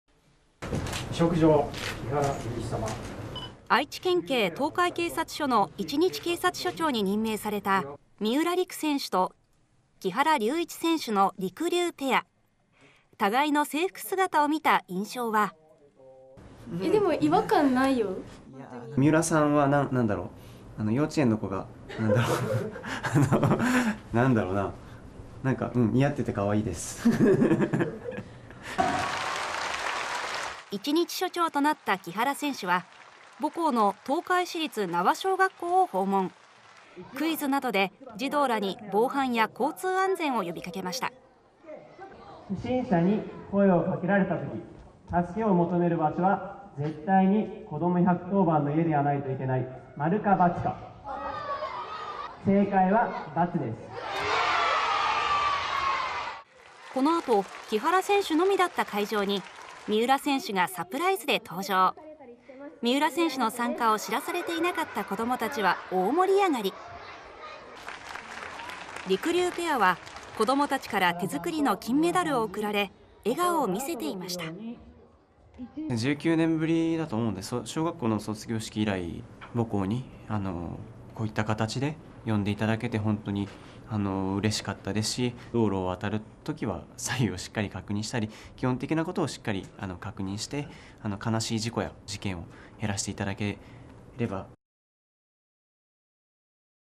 警察官姿の“りくりゅう”「違和感ないよ」「似合っていてかわいい」木原龍一選手の母校で一日署長 三浦璃来選手のサプライズ登場に歓声 愛知・東海市名和小学校
愛知県警東海警察署の一日警察署長に任命された、フィギュアの三浦璃来選手と木原龍一選手のりくりゅうペア。互いの制服姿を見た印象は…。
（三浦璃来選手）「違和感ないよ」

（木原龍一選手）
「三浦さんは…幼稚園の子が（着ているような）。似合っていてかわいい」
一日署長となった木原選手は、母校の東海市立名和（なわ）小学校を訪問。クイズなどで児童らに防犯や交通安全を呼びかけました。
（木原龍一選手）
「不審者に声をかけられたとき助けを求める場所は、絶対に『こども110番』の家でなくてはいけない。〇か×か…正解は×です」

このあと木原選手のみだった会場に、三浦選手がサプライズで登場。三浦選手の参加を知らされていなかった子どもたちは大盛り上がりです。
りくりゅうペアは子どもたちから手作りの金メダルを贈られ、笑顔を見せていました。
（木原龍一選手）
「（母校は）19年ぶり。小学校の卒業式以来で、母校にこういった形で呼んでもらえてうれしい。（子どもたちには）道路を渡るときは左右をしっかり確認するとか、基本的なことを確認して、悲しい事故や事件を減らしてほしい」